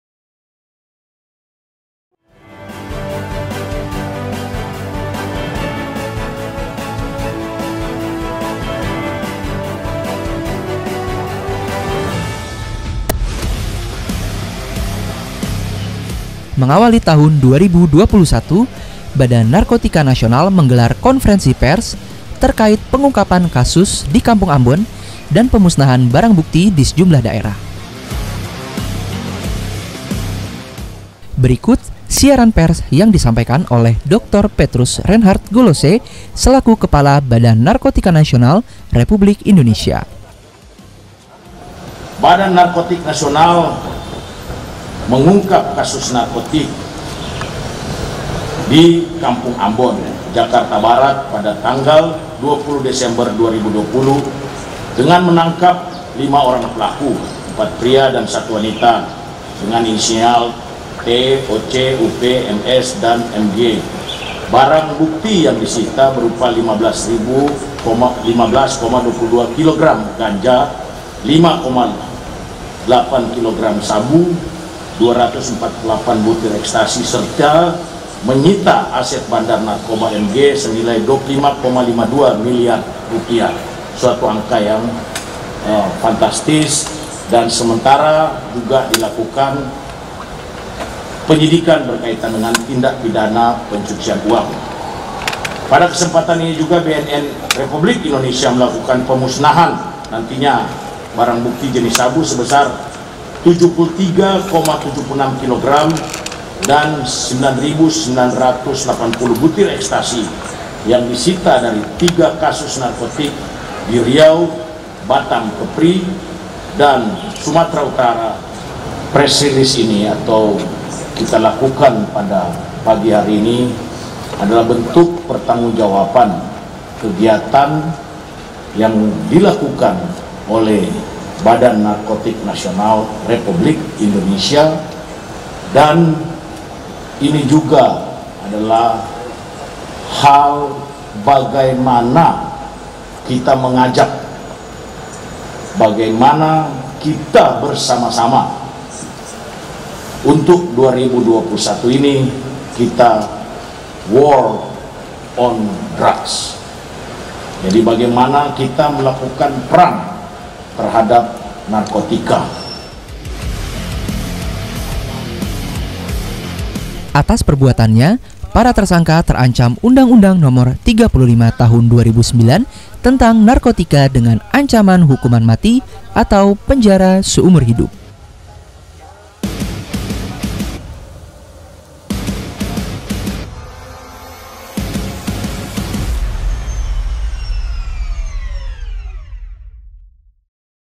BNN News : Pengungkapan Jaringan Kampung Ambon dan Pemusnahan Barang Bukti Narkotika
Badan Narkotika Nasional Republik Indonesia mengungkap kasus narkotika di Kampung Ambon, Jakarta Barat pada tanggal 20 Desember 2020 dengan menangkap 5 orang pelaku (4 pria dan 1 wanita) atas inisial T, OC, UP, MS, dan MG. barang bukti yang disita berupa 15,22 kilogram ganja; 5,8 kilogram Sabu; 248 butir ekstasi; serta menyita aset bandar narkoba MG senilai 25,52 miliar rupiah.

Pada kesempatan ini juga, BNN RI melakukan pemusnahan barang bukti narkoba jenis sabu seberat 73,76 kilogram dan 9.980 butir ekstasi yang disita dari 3 kasus narkotika di Riau, Batam Kepri, dan Sumatera Utara dengan penjelasan sebagai berikut.

1. Kasus Narkoba di Dumai, Riau
Pada tanggal 5 November 2020 petugas BNN RI melakukan pengejaran dan penangkapan pelaku SRF di wilayah Sungai Tanjung Leban, Bengkalis, Riau serta menangkap pengendalinya atas nama Ricky alias Ninja di Lapas Bengkalis Riau dengan barang bukti sabu seberat 52,09 kilogram.

2. Kasus Narkoba di Batam Pada tanggal 13 November 2020, petugas BNN bekerja sama dengan Bea Cukai menangkap 3 pelaku penumpang pesawat atas nama SUB, SY,dan SH yang membawa narkoba seberat 537,5 gram yang disembunyikan dalam dubur pelaku SUB dan SY. Sedangkan untuk pelaku SH alias Mong ditangkap di Lapas Barelang.

3. Kasus Narkoba di Kabupaten Batubara, Sumatera Utara

Pada tanggal 30 November 2020, petugas BNN mengamankan FER, BAM dan SYAF di sebuah warung makan di jalan lintas Sumatera, Perkebunan Sei Bejangkar, Kabupaten Batubara, Sumatera Utara dan berhasil melakukan penggeledahan dan penyitaan sabu seberat 20 kilogram dan ekstasi sebanyak 10.000 butir yang disembunyikan dalam cover pintu mobil.

Biro Humas dan Protokol BNN

Twitter. : @infobnn
Facebook Fan page : @humas.bnn